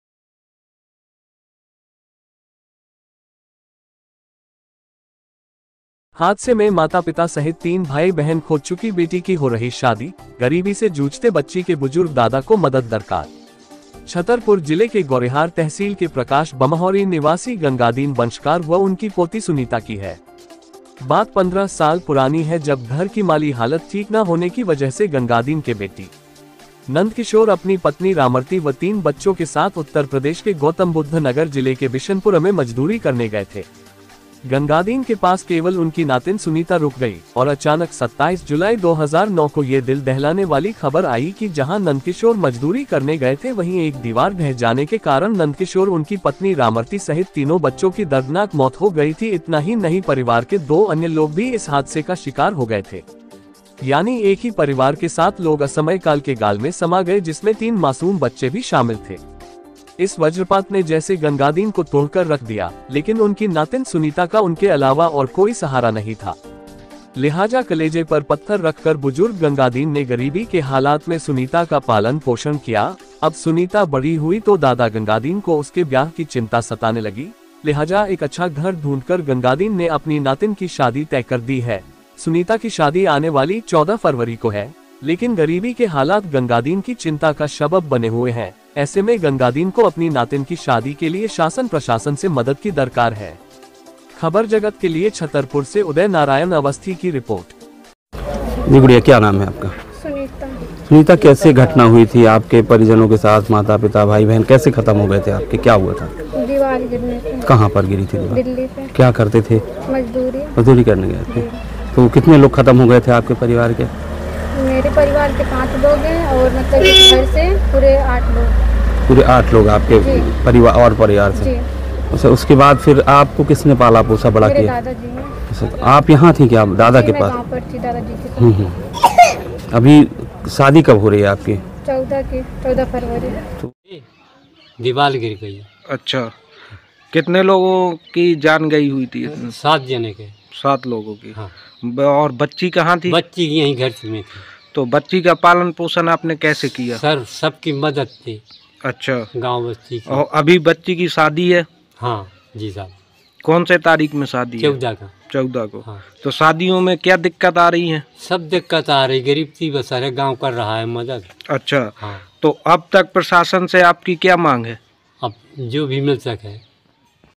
हादसे में माता पिता सहित तीन भाई बहन खो चुकी बेटी की हो रही शादी ,गरीबी से जूझते बच्ची के बुजुर्ग दा
हादसे में माता पिता सहित तीन भाई बहन खो चुकी बेटी की हो रही शादी ,गरीबी से जूझते बच्ची के बुजुर्ग दादा को मदद दरकार